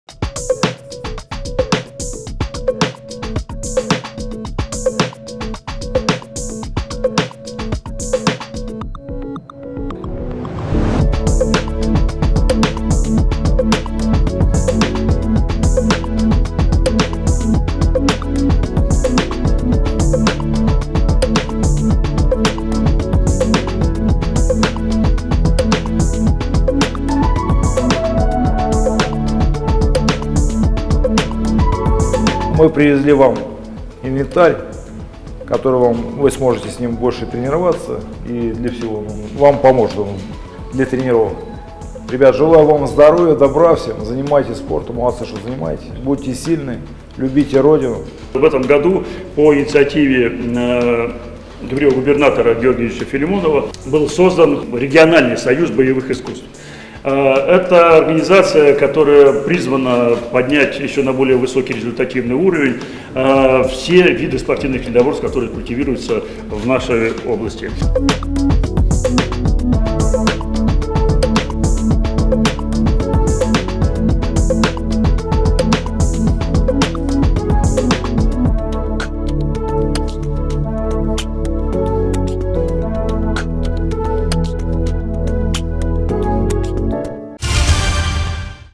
Новые тренажеры получил череповецкий Центр боевых искусств
Юные спортсмены теперь могут разминаться перед тренировками на аэробайках. Обновленный инвентарь поможет проработать все группы мышц. Сегодня 5 современных тренажеров передал Центру заместитель Губернатора Александр Поветкин. В этом году был создан региональный союз боевых искусств, призванный поднять на еще более высокий уровень все виды спортивных единоборств, которые культивируются в нашей области. Спортивный инвентарь поступит до конца августа и в другие спортивные федерации по единоборствам Вологодчины. Средства на оснащение выделены по поручению врио Губернатора Вологодской области Георгия Филимонова.